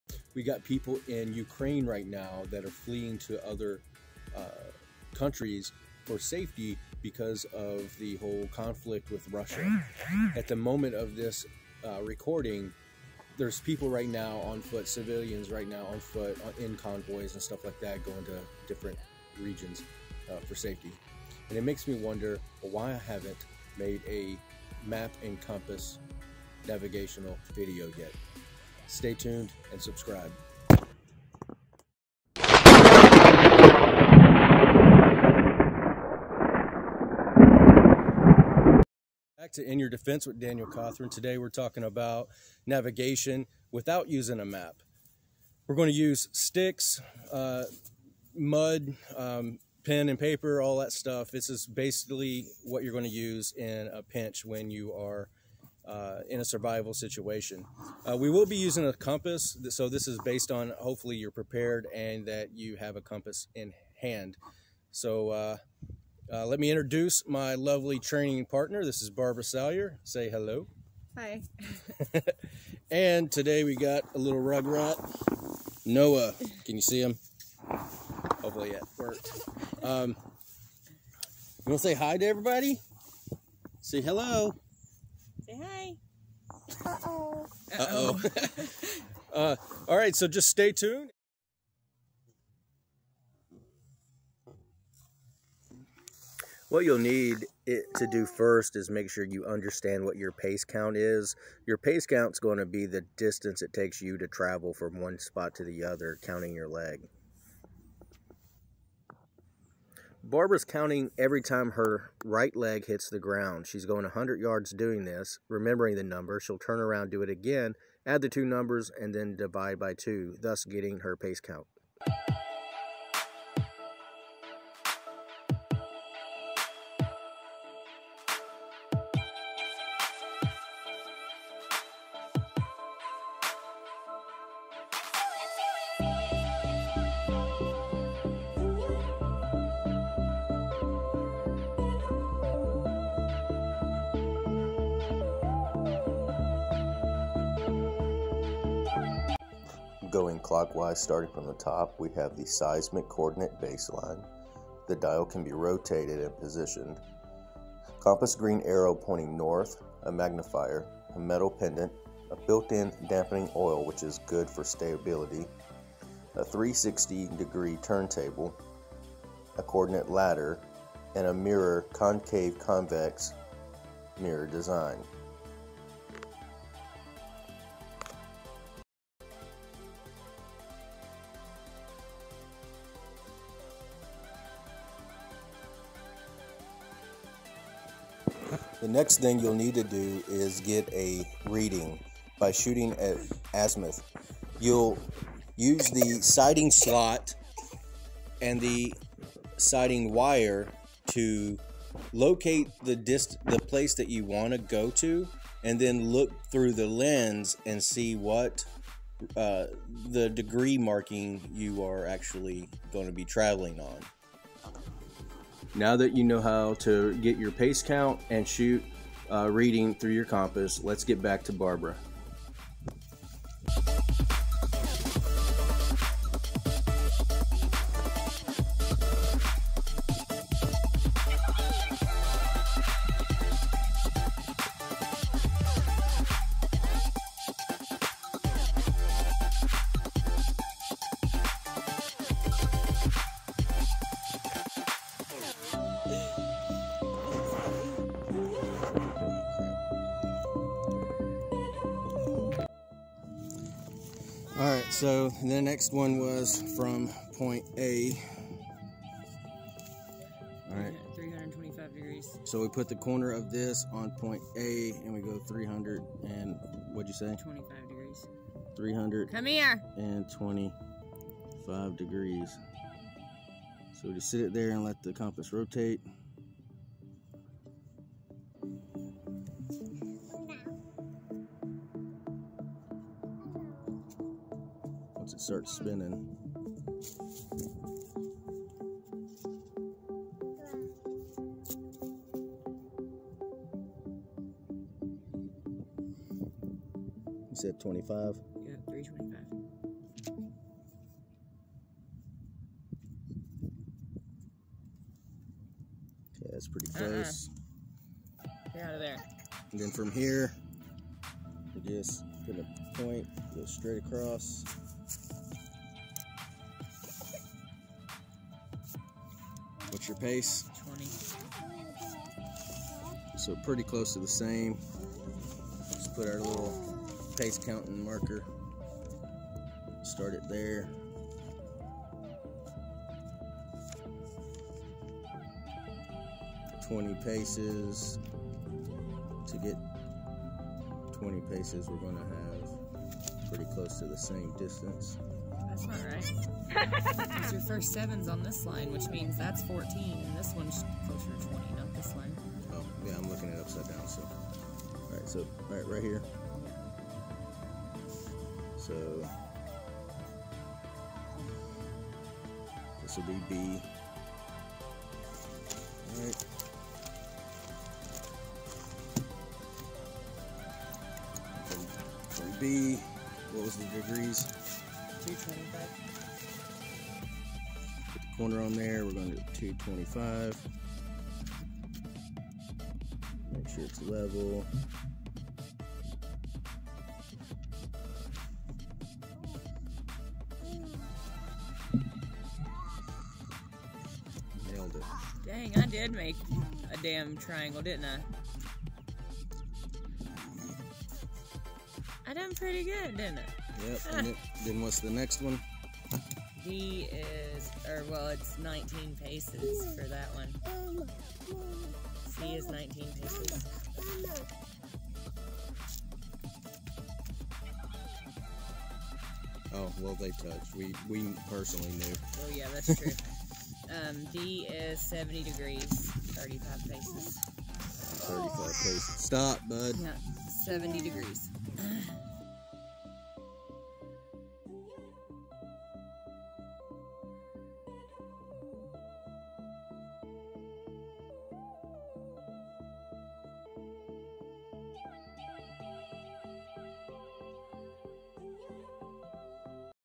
Learn to make your own map using a compass
in this video Barbara and I show you how to use a compass to create your own map in the event that you need to travel on foot